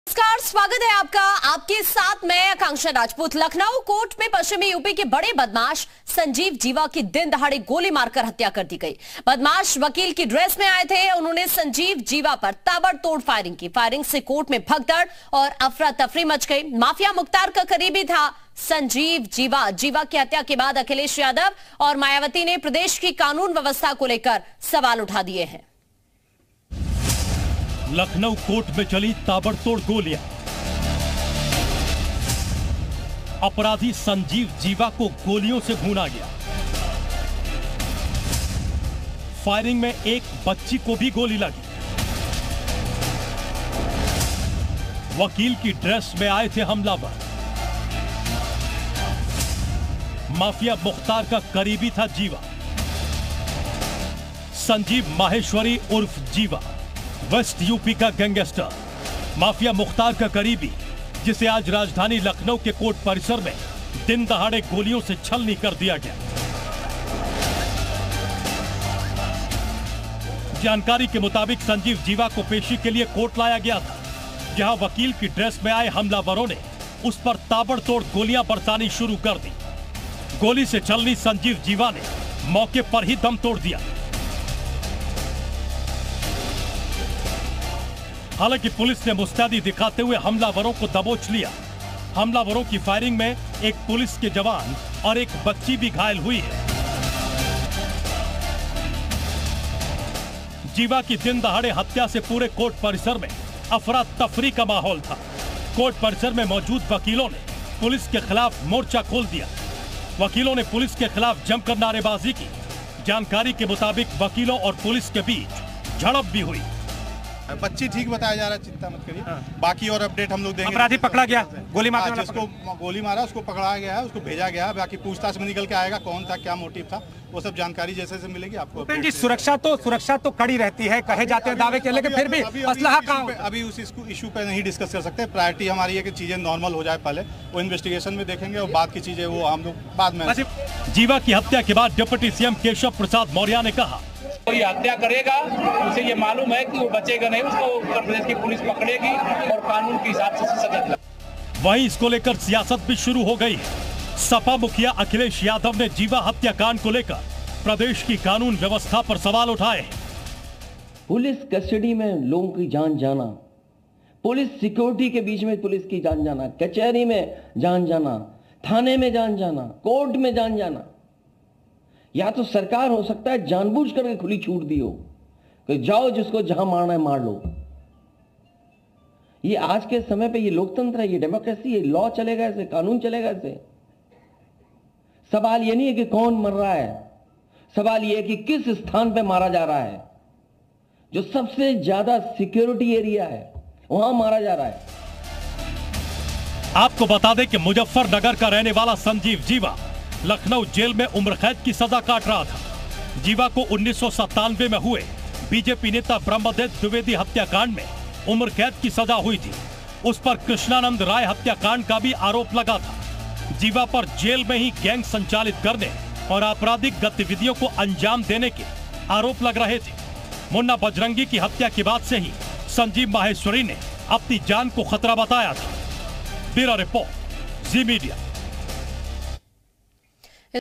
Lucknow : गैंगस्टर Sanjiv Jiva को गोलियों से भूना, वकील की ड्रेस में आए थे हमलावर | News Update
Lucknow : गैंगस्टर संजीव जीवा को गोलियों से भूना, वकील की ड्रेस में आए थे हमलावर, माफिया मुख्तार का करीबी था जीवा, यूपी के टॉप बदमाशों के लिस्ट में शामिल था जीवा